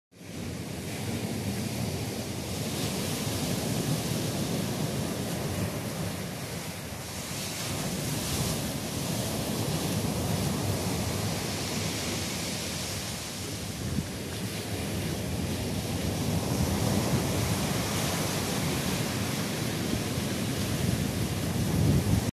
The waves at sunset.
Eastbourne East Sussex seafront at sunset on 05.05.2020.